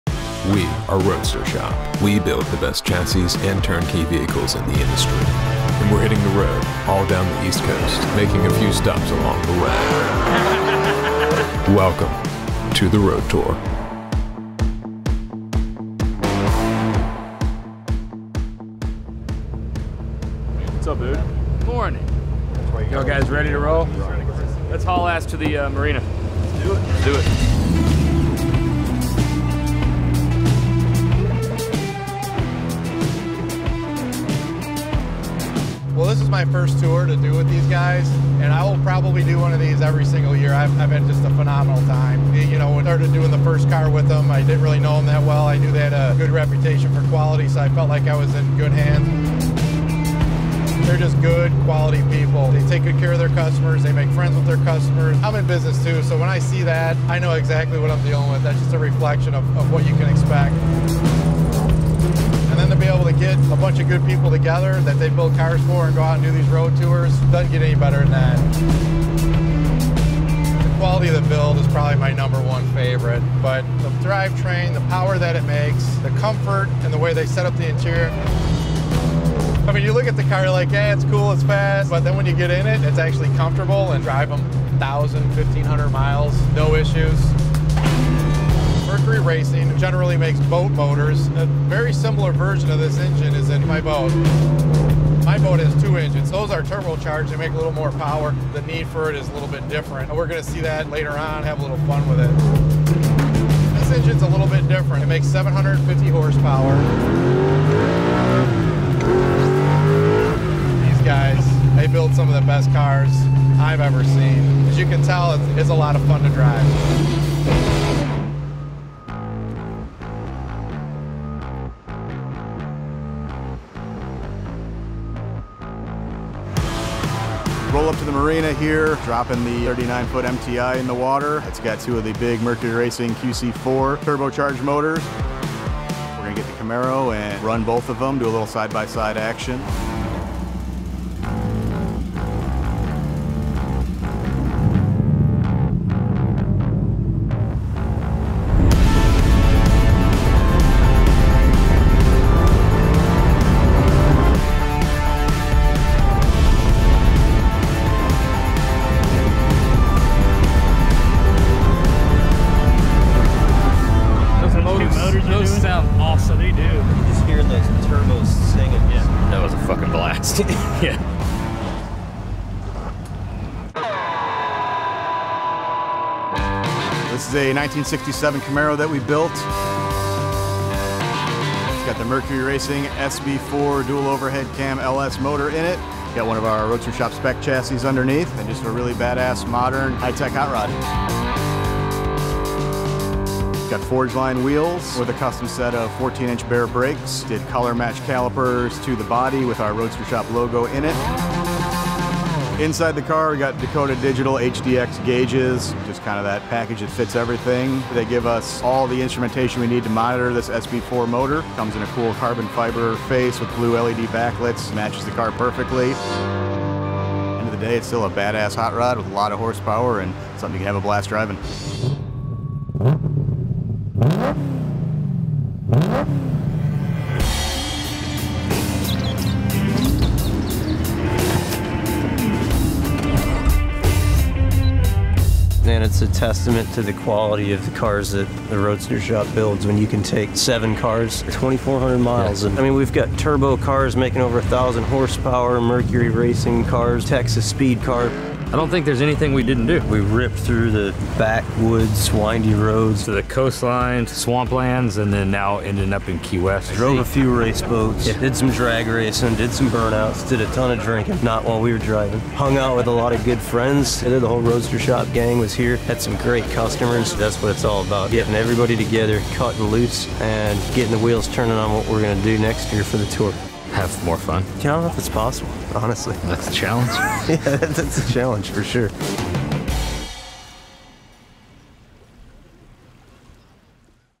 Wet and Wild in the ‘67 Camaro -The Roadster Shop: Road Tour, Ep. 5
This season on the Road Tour, you’re invited to jump on board an East Coast road trip, as the Roadster Shop team tests out some of their latest creations on the open road, visiting a few friends along the way.

For more than 25 years, the legendary Roadster Shop have been industry-leading chassis fabrication experts, and have built some truly incredible turn-key muscle cars.

This episode features one of America’s meanest creations: The ‘67 Camaro--with a Mercury Racing SB4 7.0.

Hit the streets--and the waterways--of Miami as the crew open things up in their street rods and an MTI Boat powered by Mercury Racing QC4 turbo-charged motor.

Special thanks to DEI - Design Engineering, Inc., Vintage Air, Summit Racing, Forgeline Motorsports, Edelbrock USA, Dakota Digital, HP Tuners, Hyperco,  Baer Breaks and Howler Bros.